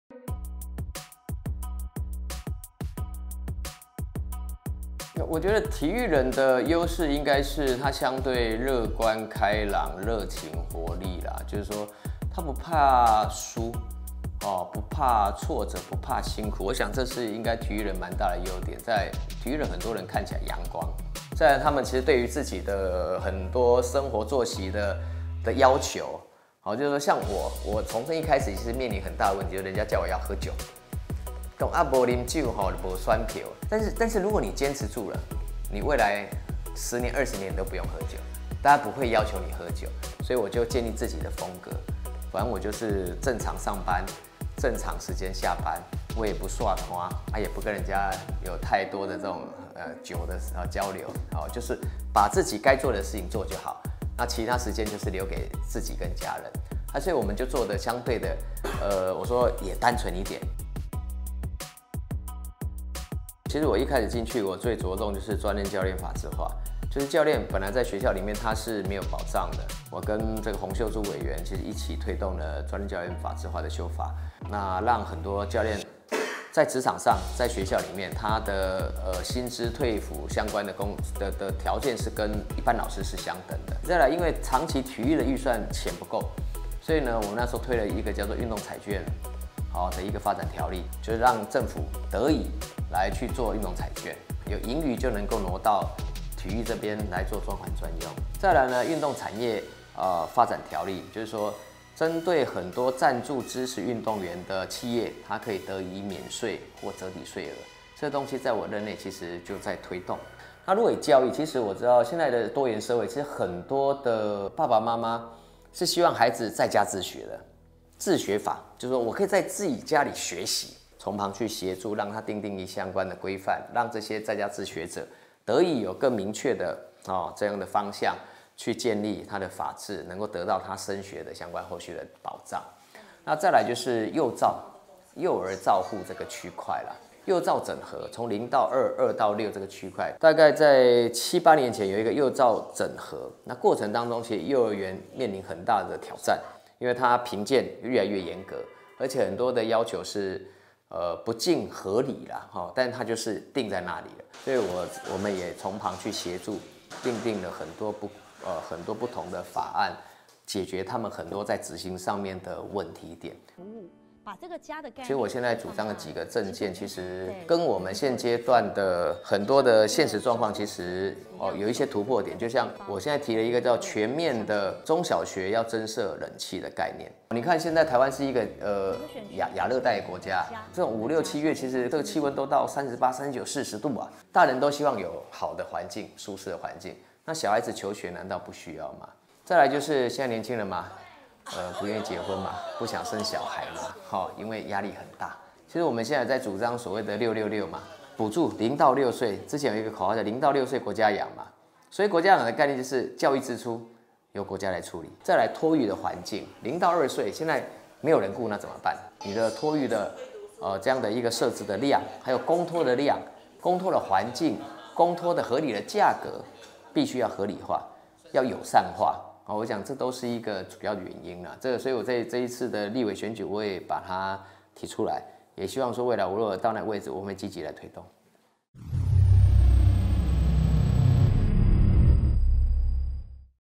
《跆拳道》體育人從政優勢 樂觀開朗不怕輸
本週星期人物 黃志雄 系列報導今天來到最後一集，談到他近年在政壇的心得。其實過去也有臺灣頂尖運動員從政的例子，例如早期的「飛躍羚羊」紀政、「亞洲鐵人」楊傳廣都當過立法委員。

而「籃球博士」 鄭志龍 、「「漂亮寶貝」」 錢薇娟 ，分別當過立法委員和前行政院體育委員會副主委，前網球國手 陳宜 現在為基隆市議員，陳宜的哥哥陳迪還是現役網球選手。

黃志雄認為，運動員早已習慣勝負，特質就是樂觀開朗、熱情活力，而且不怕輸，以及對於生活作息的規律。

他以自己為例，剛開始從政，不太習慣跑攤應酬，但他建立自己的風格，「不跟人家有太多酒的交流」、「堅持住了（原則），未來10年、20年都不用喝酒」。黃志雄固定時間陪家人，讓自己從政的工作盡量單純，努力為體育、教育、地方發聲。